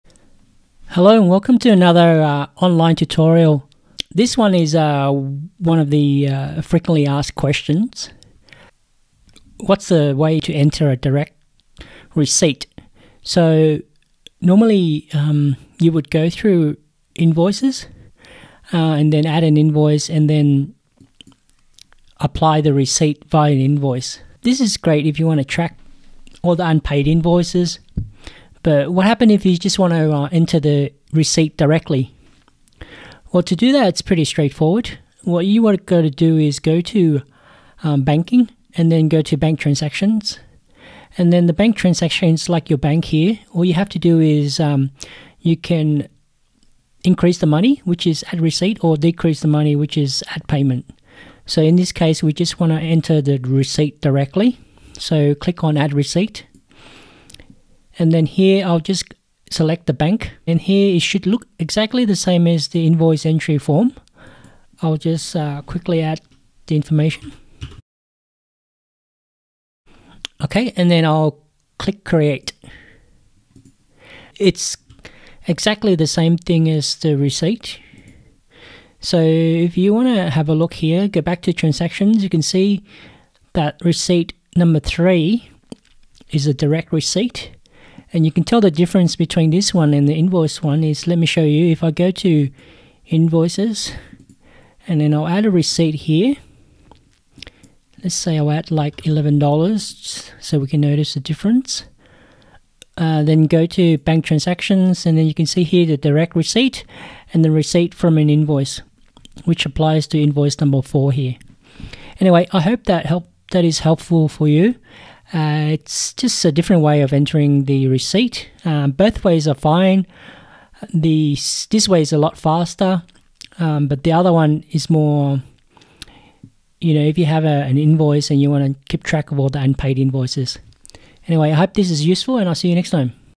How to create a receipt (without invoicing)
Another commonly asked question - How to enter a receipt directly without going through invoicing.